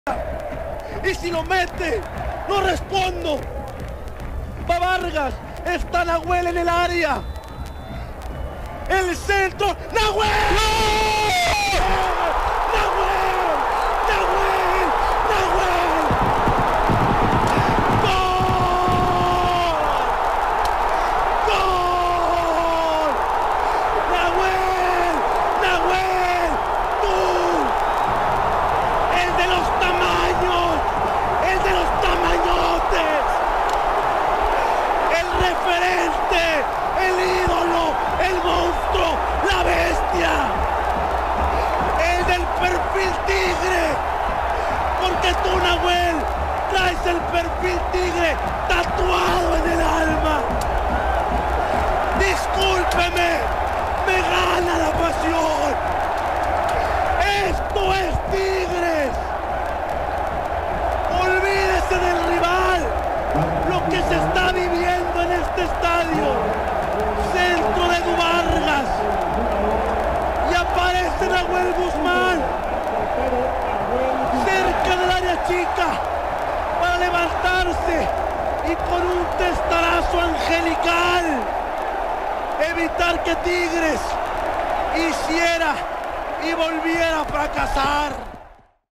Gol Nahuel Guzmán - Narración RG690 - Pello Maldonado
TIGRES 4 - 2 ALIANZA, 1/8 FINAL VUELTA
RG690 - FOXSPORTS